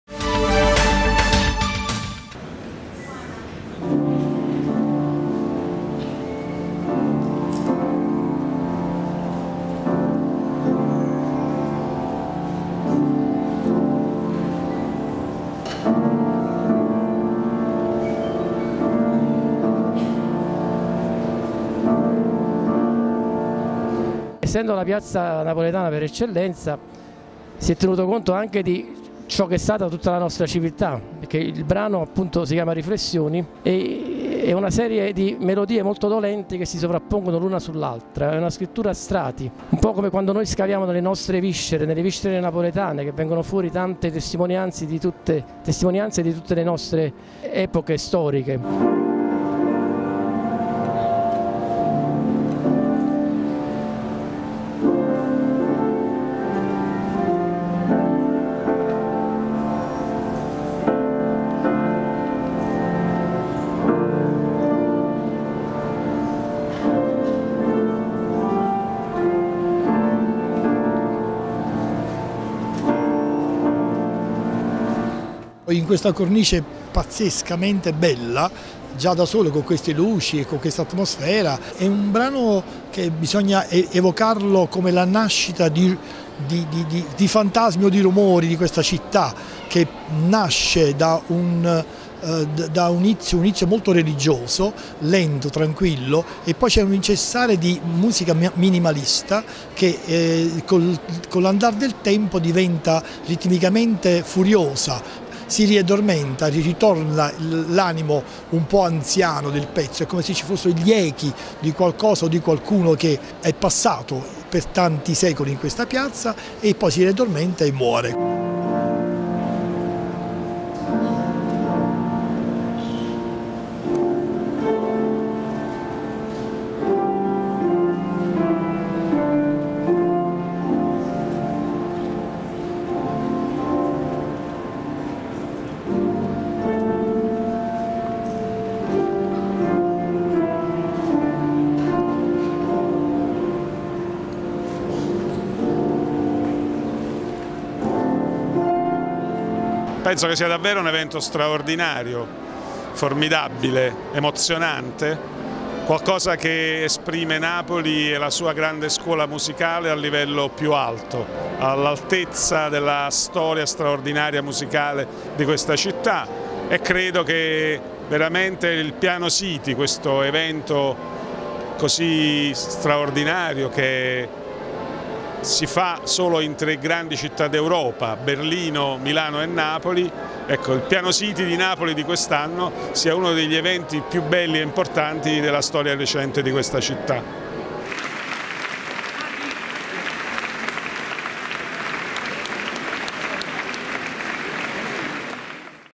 Piano City Napoli 2016: concerto di apertura in Piazza del Plebiscito
21 pianoforti suonano sotto al colonnato di San Francesco di Paola: la piazza più famosa della città ospita il concerto di apertura del Festival Piano City Napoli 2016.